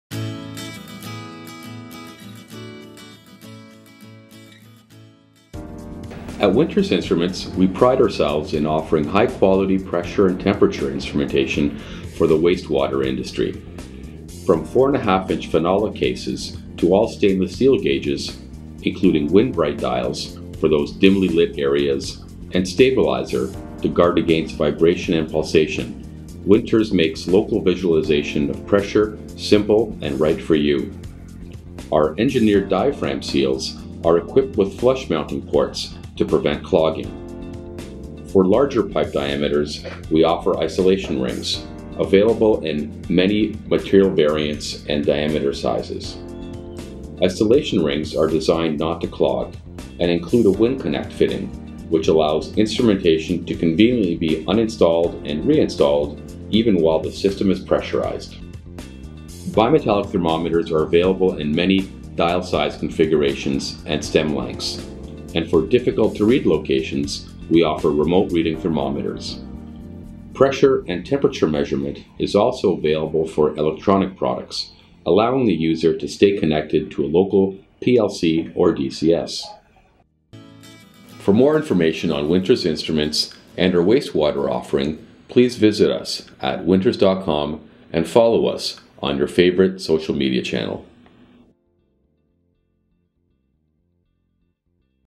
Waste Water Instrumentation
Winters Instruments is proud to present our high-quality pressure and temperature instrumentation for the waste water industry.  For more information view our waste water brochure